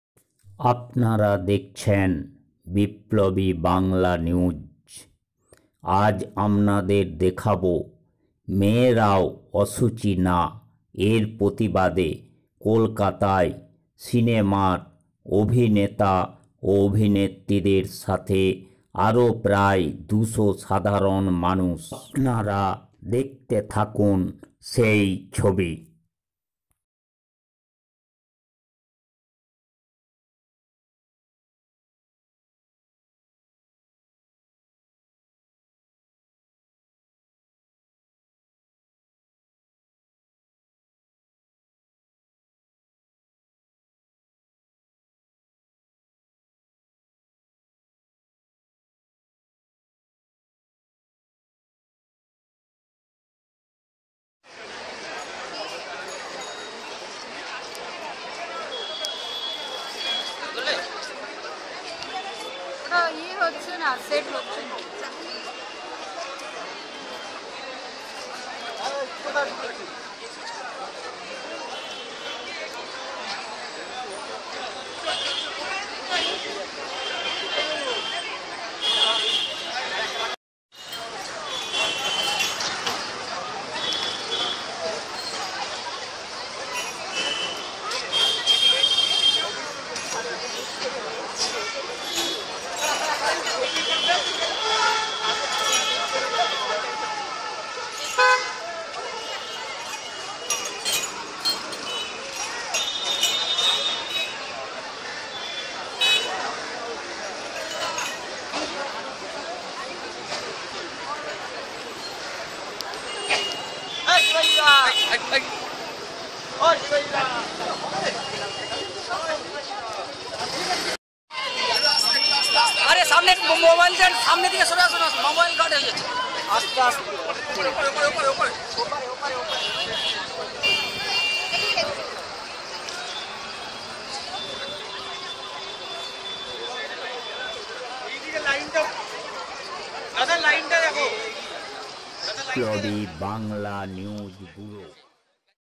মেয়েরাও অশুচি না=প্রতিবাদে সিনেমার অভিনেতা অভিনেত্রীরা কলকাতায়। বিপ্লবী বাংলা নিউজ।।
মেয়েরাও অশুচি না প্রতিবাদে সিনেমার অভিনেতা অভিনেত্রীদের সঙ্গে আরো দুজন সাধারণ মানুষ। ছয়ই মার্চ নারী দিবসে শুভ মুক্তি পাবে =ব্রহ্মা জানেন গোপন কম্ম টি । এই ছবির মধ্য দিয়ে পরিচালক সমাজের কাছে অভিনেতা-অভিনেত্রীরা বার্তা তুলে ধরতে চেয়েছেন=মেয়েরাও অশুচি না।